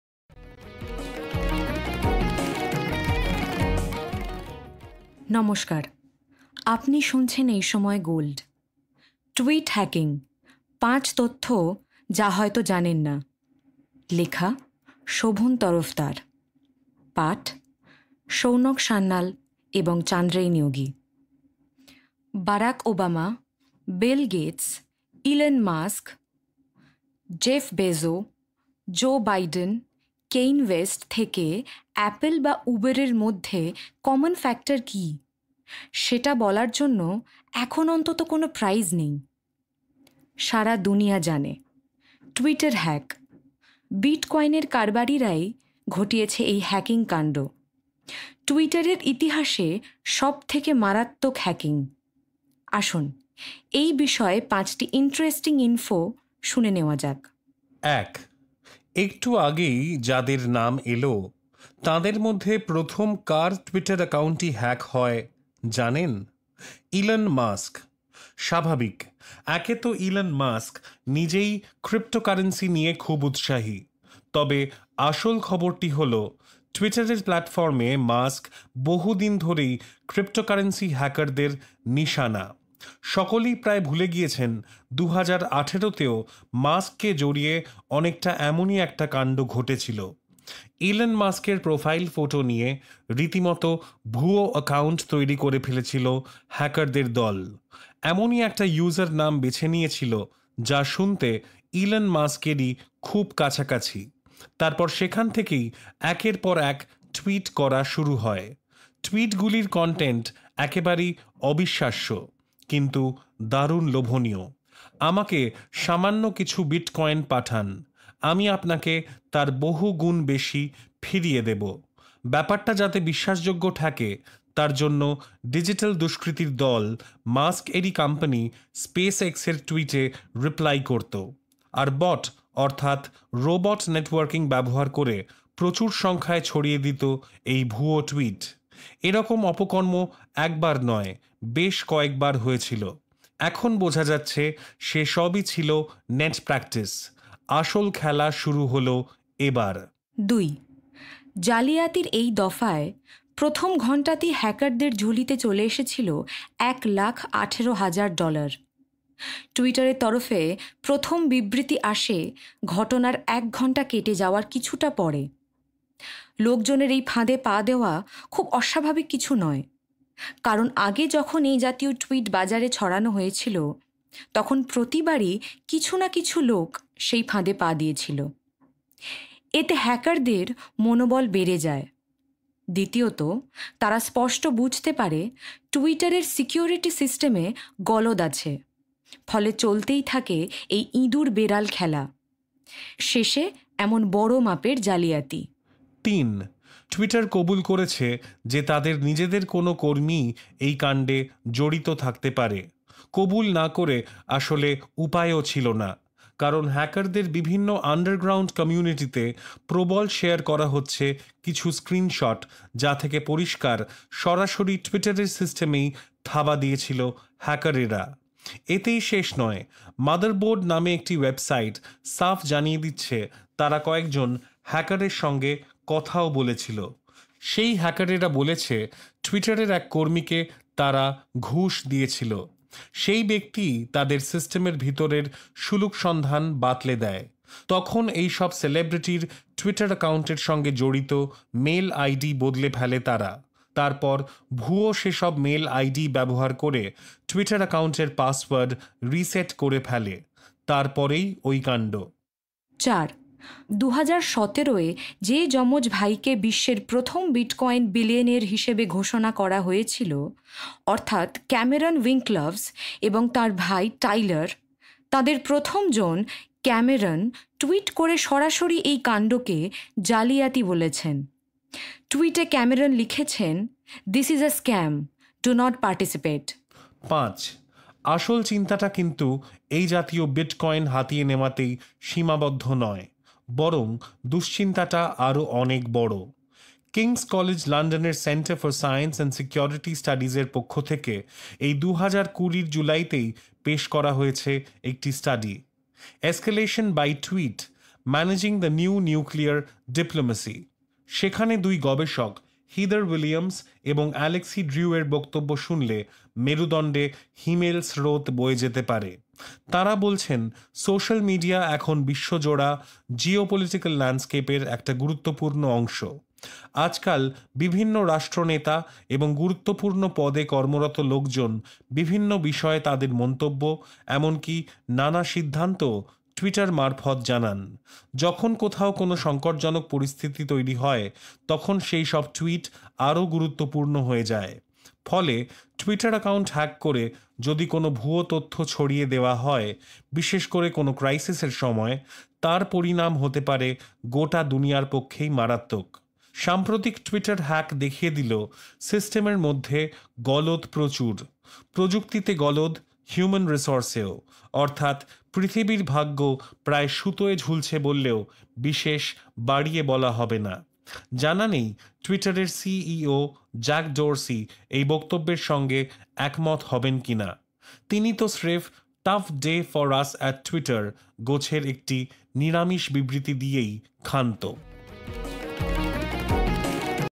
টুইট হ্যাকিং: ৫ তথ্য যা হয়তো জানেন না
টুইটার হ্যাক। বারাক ওবামা, বিল গেটস, ইলন মাস্ক...— সকলেরই হয়েছিল। বলা যায়, বিটকয়েনের কারবারিরাই ঘটিয়েছে এই হ্যাকিং-কাণ্ড। কিন্তু কী ভাবে?

শোভন তরফদার

বারাক ওবামা, বিল গেটস, ইলন মাস্ক, জেফ বেজোস, জো বাইডেন, কেইন ওয়েস্ট থেকে অ্যাপল বা উবর-এর মধ্যে কমন ফ্যাক্টর কী, সেটা বলার জন্য এখন অন্তত কোনও প্রাইজ নেই। সারা দুনিয়া জানে। টুইটার হ্যাক। বিটকয়েনের কারবারিরাই ঘটিয়েছে এই হ্যাকিং-কাণ্ড। টুইটারের ইতিহাসে সবথেকে মারাত্মক হ্যাকিং। আসুন, এই বিষয়ে পাঁচটি ইন্টারেস্টিং ইনফো শুনে নেওয়া যাক।

এক, একটু আগেই যাঁদের নাম এল, তাঁদের মধ্যে প্রথম কার টুইটার অ্যাকাউন্টটি হ্যাক হয়, জানেন? ইলন মাস্ক। স্বাভাবিক। একে তো ইলন মাস্ক নিজেই ক্রিপ্টোকারেন্সি নিয়ে খুব উৎসাহী। তবে, আসল খবরটি হল টুইটারের প্ল্যাটফর্মে মাস্ক বহু দিন ধরেই ক্রিপ্টোকারেন্সি হ্যাকারদের নিশানা। সকলেই প্রায় ভুলে গিয়েছেন, ২০১৮-তেও মাস্ককে জড়িয়ে অনেকটা এমনই একটা কাণ্ড ঘটেছিল। ইলন মাস্কের প্রোফাইল ফোটো নিয়ে রীতিমতো ভুয়ো অ্যাকাউন্ট তৈরি করে ফেলেছিল হ্যাকারদের দল, এমনই একটা ইউজার নাম বেছে নিয়েছিল যা শুনতে ইলন মাস্ক-এরই খুব কাছাকাছি, তারপর সেখান থেকেই একের পর এক টুইট করা শুরু হয়। টুইটগুলির কনটেন্ট একেবারেই অবিশ্বাস্য, কিন্তু দারুণ লোভনীয়। আমাকে সামান্য কিছু বিটকয়েন পাঠান, আমি আপনাকে তার বহু গুণ বেশি ফিরিয়ে দেব। ব্যাপারটা যাতে বিশ্বাসযোগ্য ঠেকে, তার জন্য ডিজিটাল দুষ্কৃতীর দল মাস্ক-এরই কোম্পানি স্পেস এক্স-এর টুইটে রিপ্লাই করত। আর, বট, অর্থাৎ রোবট নেটওয়ার্কিং ব্যবহার করে প্রচুর সংখ্যায় ছড়িয়ে দিত এই ভুয়ো টুইট। এ রকম অপকর্ম একবার নয়, বেশ কয়েক বার হয়েছিল। এখন বোঝা যাচ্ছে, সে সবই ছিল নেট প্র্যাকটিস। আসল খেলা শুরু হল এ বার।

দুই, জালিয়াতির এই দফায় প্রথম ঘণ্টাতেই হ্যাকারদের ঝুলিতে চলে এসেছিল এক লাখ আঠেরো হাজার ডলার। টুইটারের তরফে প্রথম বিবৃতি আসে ঘটনার এক ঘণ্টা কেটে যাওয়ার কিছুটা পরে। লোকজনের এই ফাঁদে পা দেওয়া খুব অস্বাভাবিক কিছু নয়, কারণ, আগে যখন এই জাতীয় টুইট বাজারে ছড়ানো হয়েছিল, তখন প্রতিবারই কিছু না কিছু লোক সেই ফাঁদে পা দিয়েছিল। এতে হ্যাকারদের মনোবল বেড়ে যায়। দ্বিতীয়ত, তারা স্পষ্ট বুঝতে পারে, টুইটারের সিকিওরিটি সিস্টেমে গলদ আছে। ফলে, চলতেই থাকে এই ইঁদুর-বেড়াল খেলা। শেষে এমন বড় মাপের জালিয়াতি।

তিন, টুইটার কবুল করেছে যে তাদের নিজেদের কোনও কর্মীই এই কাণ্ডে জড়িত থাকতে পারে। কবুল না করে আসলে উপায়ও ছিল না, কারণ হ্যাকারদের বিভিন্ন আন্ডারগ্রাউন্ড কমিউনিটিতে প্রবল শেয়ার করা হচ্ছে কিছু স্ক্রিনশট, যা থেকে পরিষ্কার, সরাসরি টুইটারের সিস্টেমেই থাবা দিয়েছিল হ্যাকারেরা। এতেই শেষ নয়, মাদারবোর্ড নামে একটি ওয়েবসাইট সাফ জানিয়ে দিচ্ছে, তারা কয়েকজন হ্যাকারের সঙ্গে কথাও বলেছিল। সেই হ্যাকারেরা বলেছে টুইটারের এক কর্মীকে তারা ঘুষ দিয়েছিল। সেই ব্যক্তিই তাদের সিস্টেমের ভিতরের সুলুকসন্ধান বাতলে দেয়। তখন এই সব সেলিব্রিটির টুইটার অ্যাকাউন্টের সঙ্গে জড়িত মেল আইডি বদলে ফেলে তারা, তারপর ভুয়ো সে সব মেল আইডি ব্যবহার করে টুইটার অ্যাকাউন্টের পাসওয়ার্ড রিসেট করে ফেলে। তারপরেই ওই কাণ্ড।

চার, ২০১৭-য় যে যমজ ভাইকে বিশ্বের প্রথম বিটকয়েন বিলিয়নেয়ার হিসাবে ঘোষণা করা হয়েছিল, অর্থাৎ ক্যামেরন উইঙ্কলভস এবং তাঁর ভাই টাইলার, তাঁদের প্রথম জন, ক্যামেরন টুইট করে সরাসরি এই কাণ্ডকে ‘জালিয়াতি’ বলেছেন। টুইটে ক্যামেরন লিখেছেন, ‘দিস ইজ আ স্ক্যাম, ডু নট পার্টিসিপেট!’

পাঁচ, আসল চিন্তাটা কিন্তু এই জাতীয় বিটকয়েন হাতিয়ে নেওয়াতেই সীমাবদ্ধ নয়। বরং, দুশ্চিন্তাটা আরও অনেক বড়। কিংস কলেজ লন্ডন-এর ‘সেন্টার ফর সায়েন্স অ্যান্ড সিকিওরিটি স্টাডিজ’-এর পক্ষ থেকে এই ২০২০-র জুলাইতেই পেশ করা হয়েছে একটি স্টাডি ‘এসক্যালেশন বাই টুইট: ম্যানেজিং দ্য নিউ নিউক্লিয়ার ডিপ্লোম্যাসি’। সেখানে দুই গবেষক হিদার উইলিয়মস এবং অ্যালেক্সি ড্রিউ-য়ের বক্তব্য শুনলে মেরুদণ্ডে হিমেল স্রোত বয়ে যেতে পারে। তাঁরা বলছেন, সোশ্যাল মিডিয়া এখন বিশ্বজোড়া জিও-পলিটিক্যাল ল্যান্ডস্কেপের একটা গুরুত্বপূর্ণ অংশ। আজকাল বিভিন্ন রাষ্ট্রনেতা এবং গুরুত্বপূর্ণ পদে কর্মরত লোকজন বিভিন্ন বিষয়ে তাঁদের মন্তব্য, এমনকী নানা সিদ্ধান্তও টুইটার মারফত জানান। যখন কোথাও কোনও সংকটজনক পরিস্থিতি তৈরি হয়, তখন সেই সব টুইট আরও গুরুত্বপূর্ণ হয়ে যায়। ফলে, টুইটার অ্যাকাউন্ট হ্যাক করে যদি কোনও ভুয়ো তথ্য ছড়িয়ে দেওয়া হয়, বিশেষ করে কোনও ক্রাইসিস-এর সময়, তার পরিণাম হতে পারে গোটা দুনিয়ার পক্ষেই মারাত্মক। সাম্প্রতিক টুইটার হ্যাক দেখিয়ে দিল, সিস্টেমের মধ্যে গলদ প্রচুর। প্রযুক্তিতে গলদ, হিউম্যান রিসোর্সেও। অর্থাৎ, পৃথিবীর ভাগ্য প্রায় সুতোয় ঝুলছে বললেও বিশেষ বাড়িয়ে বলা হবে না। জানা নেই, টুইটারের সিইও জ্যাক ডোরসি এই বক্তব্যের সঙ্গে একমত হবেন কি না। তিনি তো স্রেফ ‘টাফ ডে ফর আস অ্যাট টুইটার’ গোছের একটি নিরামিষ বিবৃতি দিয়েই ক্ষান্ত।